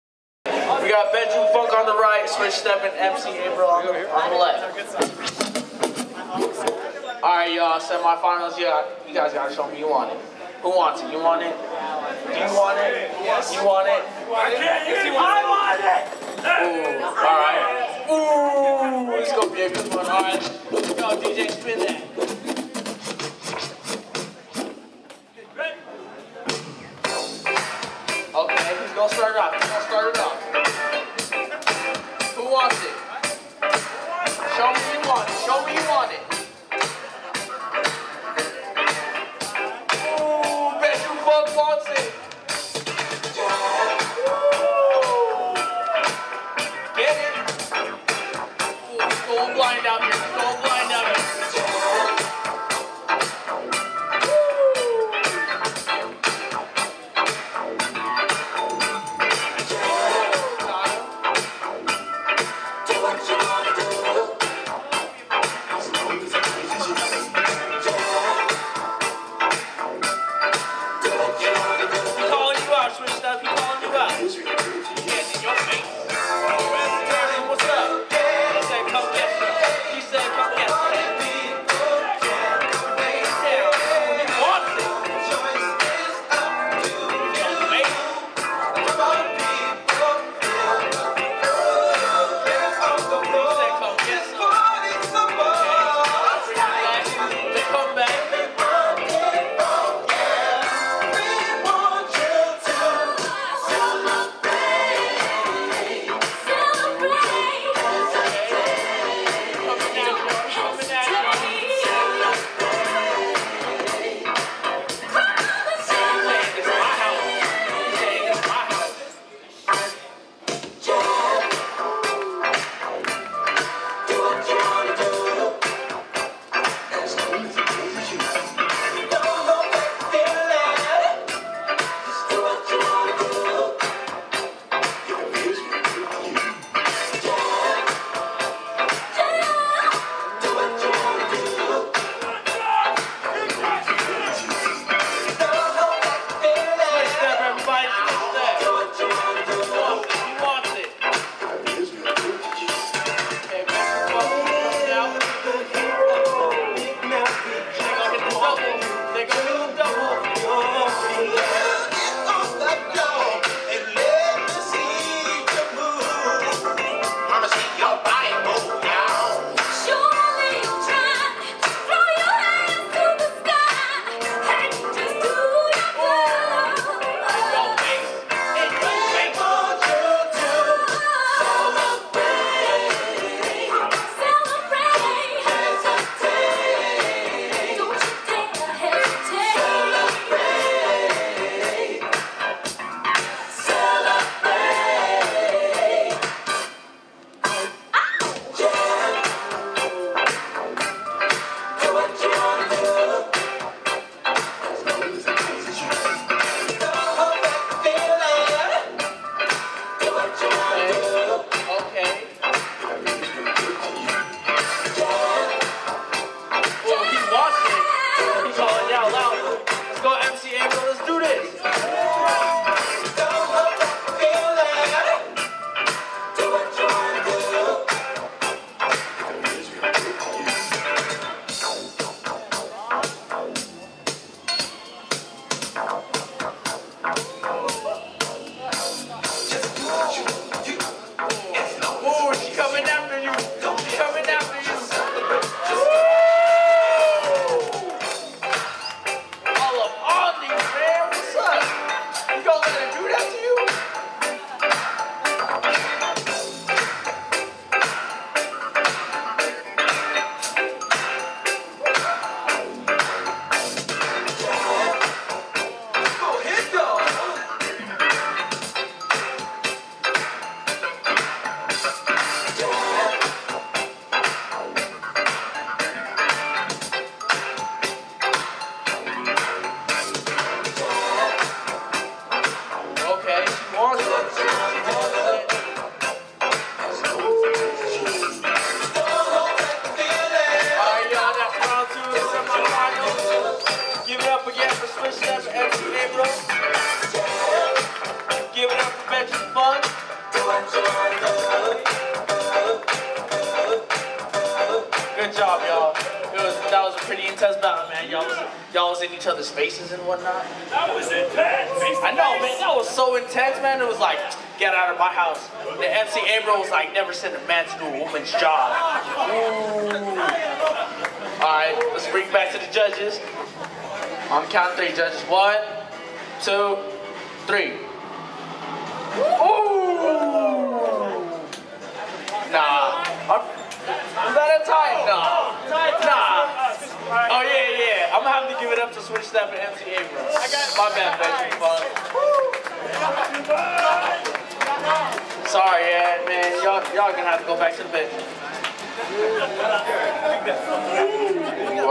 Top 4: Bedroom Funk vs SwitchStep & MC Abril | Breakers at War IV | Funk'd Up TV
Dancers - Bedroom Funk (Way & Cody) vs SwitchStep & MC Abril

Event: Breakers At War IV (2 vs 2 Locking and Popping Buddy Battle)
Date: July 23, 2011
Location: Daly City, CA, USA

*Judges*
Doc Lock (GrooveMekanex)
Polotix (Renegade Rockers)
OG Smurf